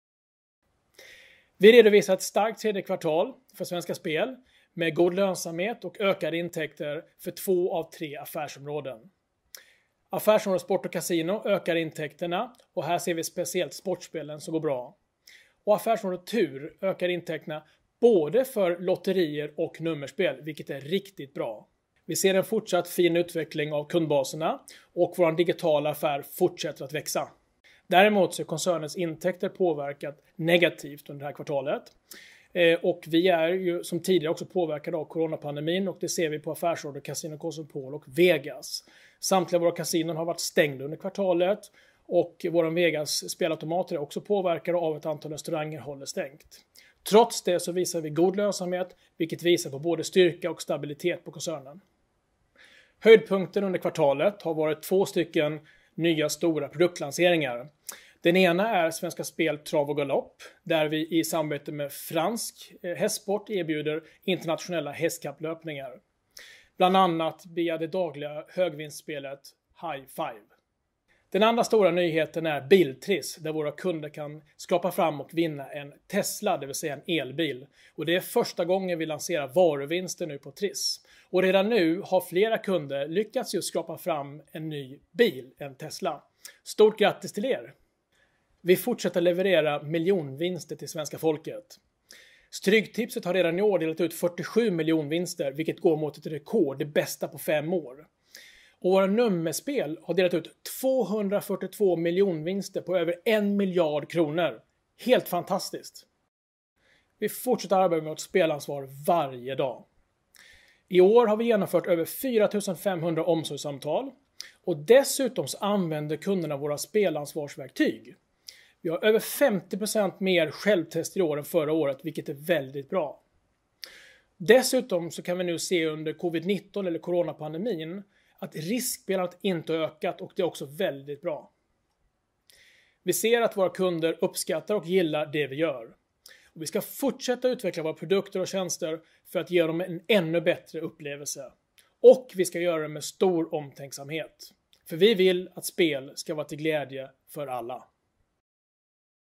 Svenska Spel - Vd och koncernchef Patrik Hofbauer om delårsrapport januari-september 2020
Svenska Spel redovisar ett starkt tredje kvartal med god lönsamhet för koncernen och ökade intäkter inom såväl sportspel som nummerspel och lotter. Det visar Svenska Spels delårsrapport januari–september 2020. Läs mer på om.svenskaspel.se/nyhetsrum/koncernnyheter
Delårsrapporten i sin helhet finns här:
om.svenskaspel.se/bolagsstyrning/finansiell-information